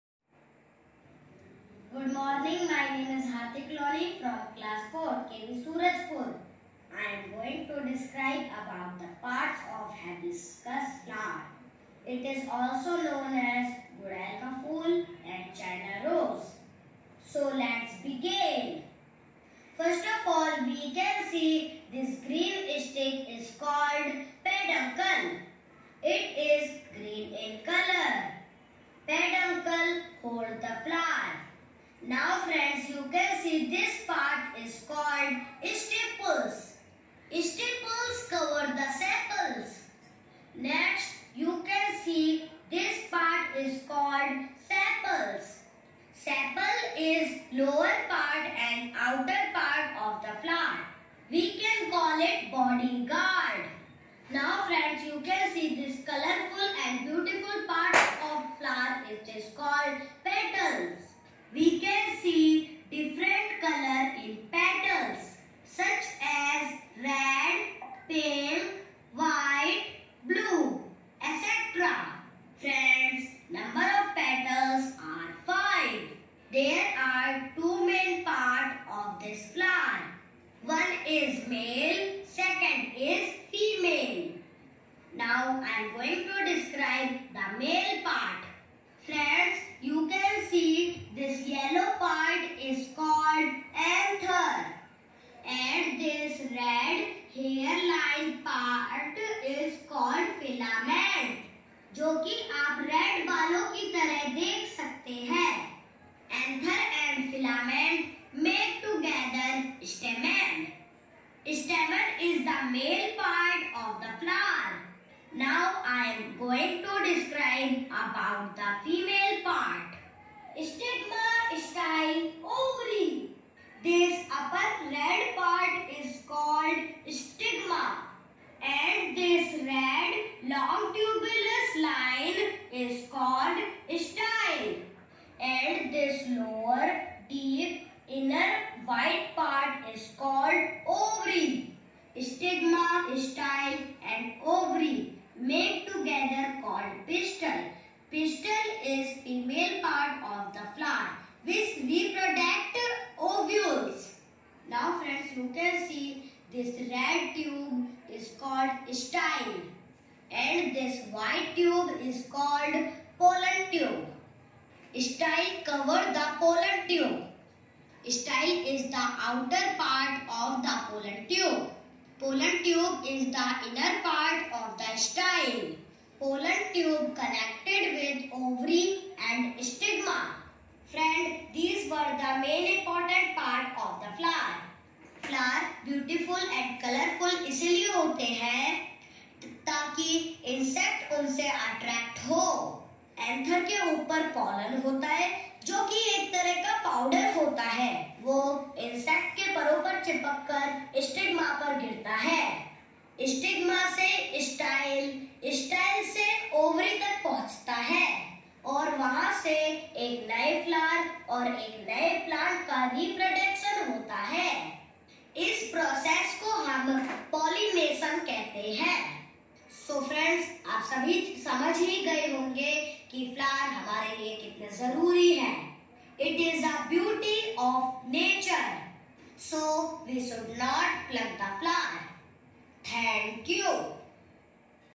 Part of Hibiscus flower - detailed explanation
Part of flower knowledge is very necessary for all, the presentation give by Hardik Lohuni.